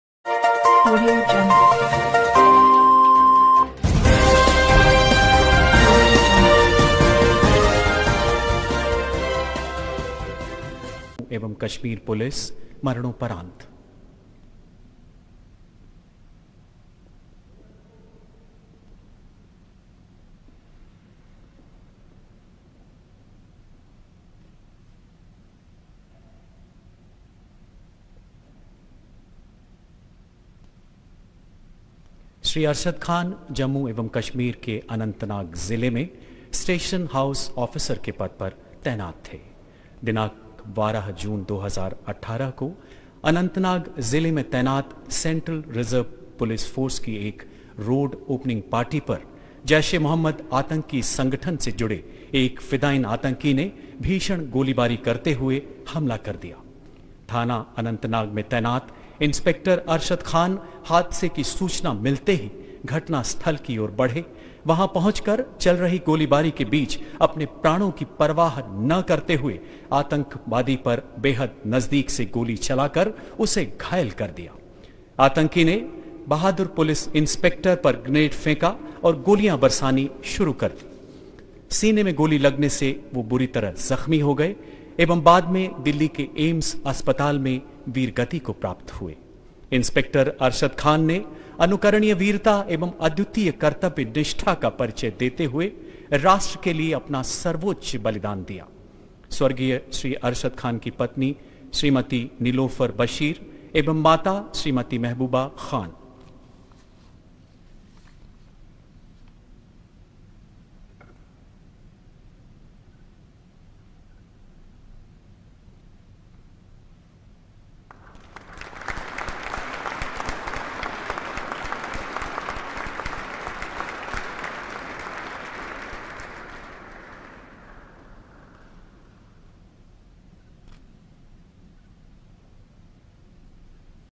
President of India Kovind Presents Shaurya Chakra to Arshad Khan (Inspector)
President of India Kovind Presents Shaurya Chakra to Arshad Khan (Inspector) Jammu and Kashmir Police (Posthumous). He displayed exemplary bravery and extra ordinary commitment towards the call of duty.